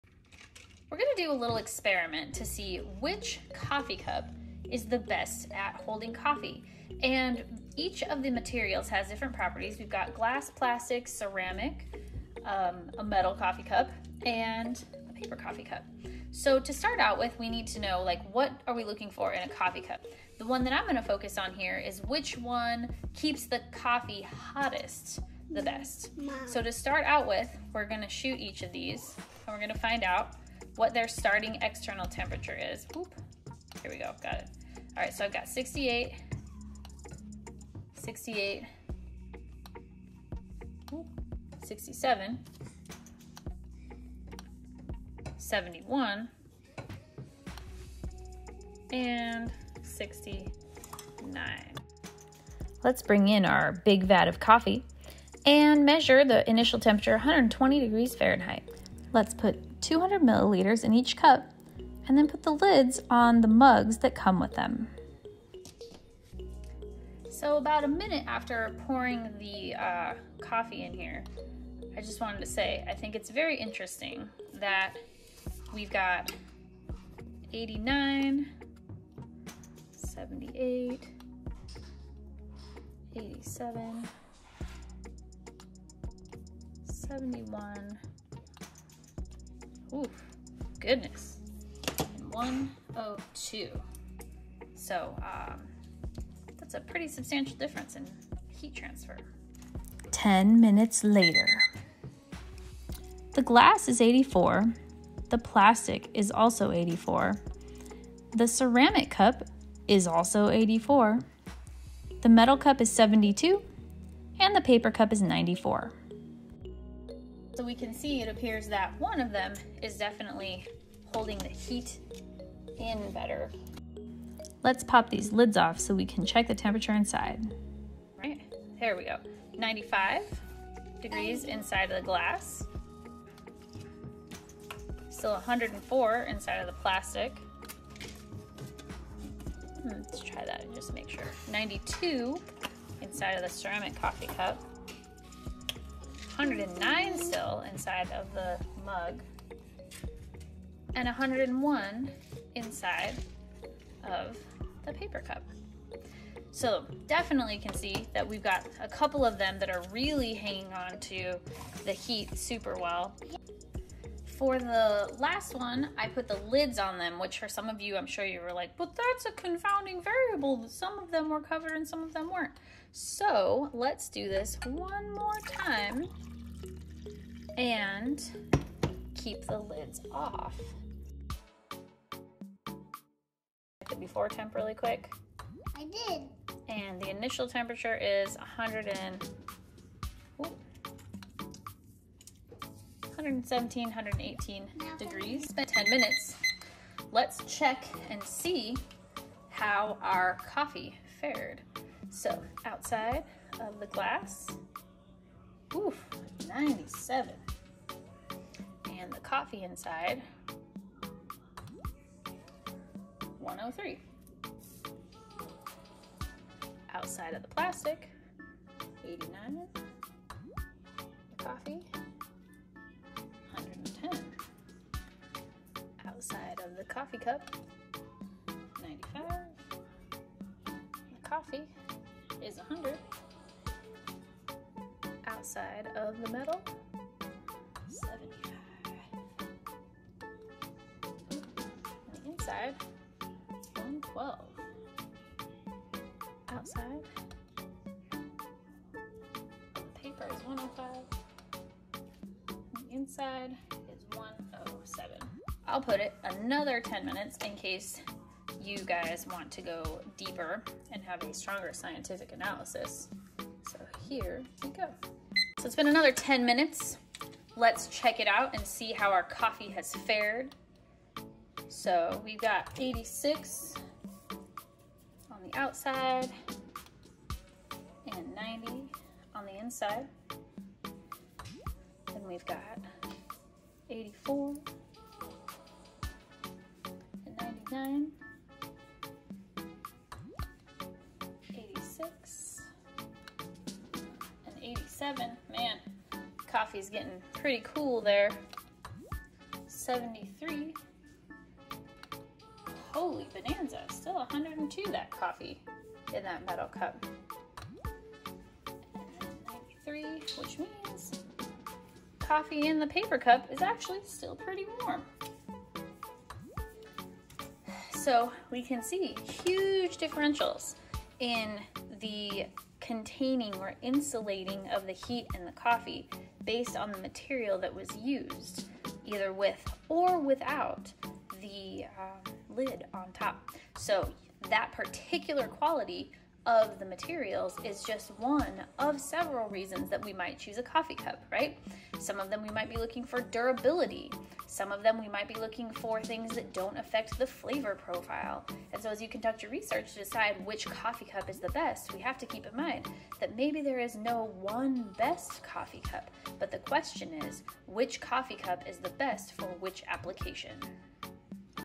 Video lab: best insulator for coffee mug
Students can observe 5 different coffee mugs being tested over 30 elapsed minutes to see which material is the best insulator. Feel free to use as an example for experimental critique.